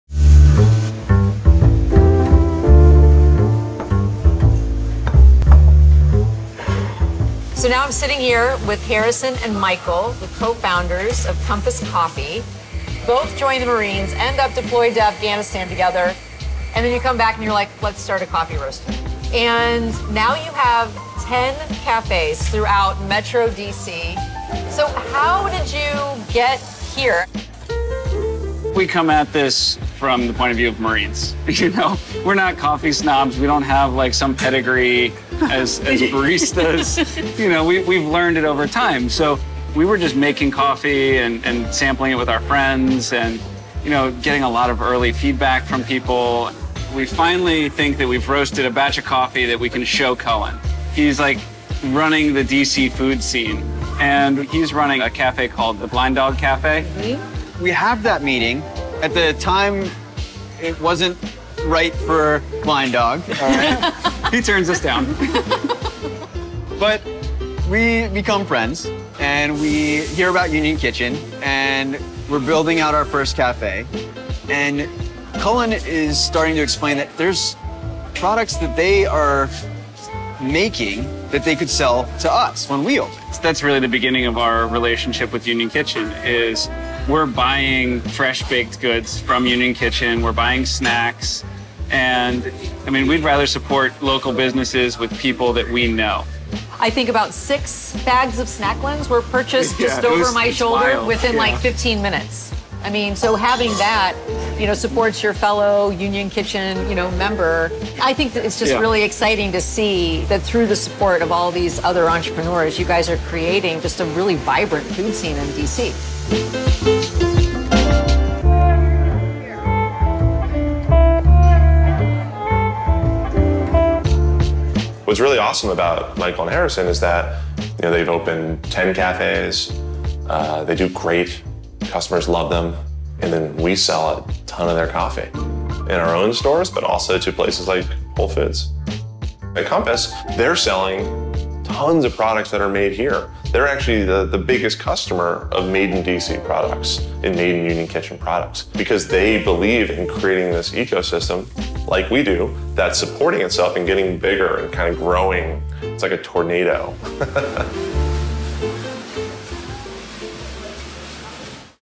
At Compass Coffee, they sell "tons" of products from Union Kitchen's makers
Compass Coffee moves a bunch of the small-batch products from Union Kitchen's Washington DC makers. “[Union Kitchen] drops off all of the goods for our cafes, then they get all the green coffee and packaging materials and bring those to us, we do the roasting and packaging, and then we sell those to Union Kitchen, which distributes to their own stores as well as stores like Whoel Foods,” says Harrison. “Through their distribution, we’re participating in this ecosystem that Cullen has built.”